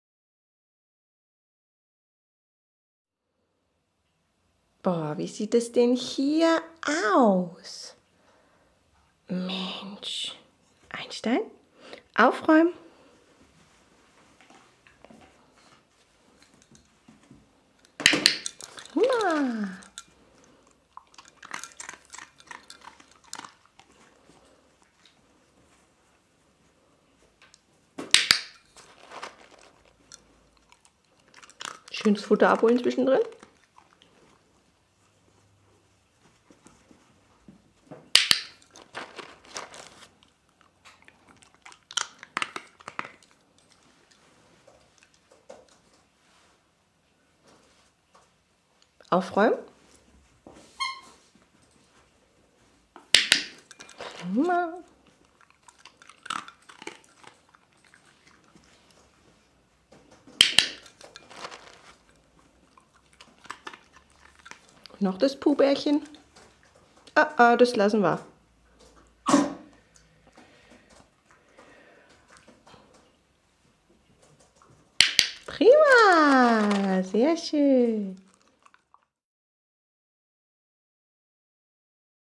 Trick Dog Einstein: Aufräumen
Einstein räumt sein Spielzeug in die Box.